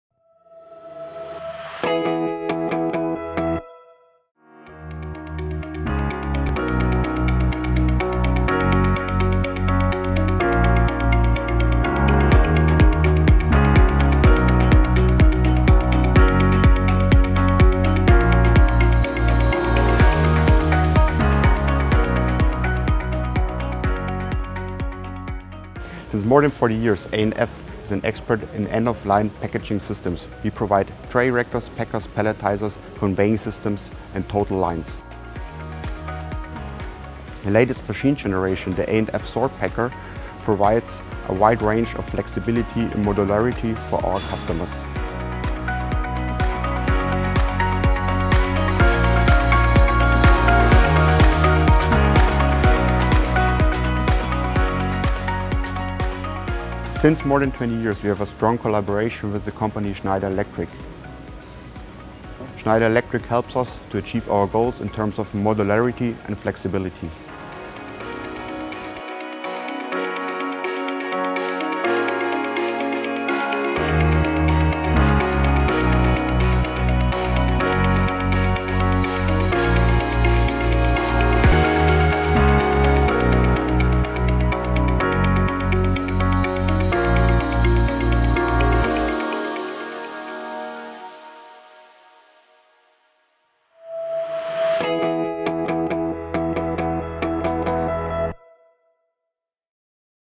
A+F Packaging Solutions - A+F SortPacker
The Sort Packer provides an alternative to manual sorting. The Sort Packer features a compact construction; it is designed as a stand-alone facility. The smallest cell is made up of two Flex Pickers and two feeding tracks. These cells are modular and extensible. Each Sort Packer can be extended to include a packaging facility. An A+F Setline can also be added.

The sorting is configurable. The first and second layers can be different when using multi-layer packaging. Various product shapes can be repackaged. Replacement gripper systems are available as an option. A variable packing configuration for bundles is also possible. Normally, products are packaged in the same bundle. However, it is also possible to pack them in plastic boxes. 12-pack bundles can also be converted to 24-pack bundles.

By default, the controller is in the machine. This ensures quick installations. The controller uses a Schneider Electric PacDrive3, or alternatively, a Rockwell controller. Prior to delivery, the A+F Sort Packer is run with original sample materials and a final inspection is documented. All pneumatic machine elements are configured, by default, with components from Festo.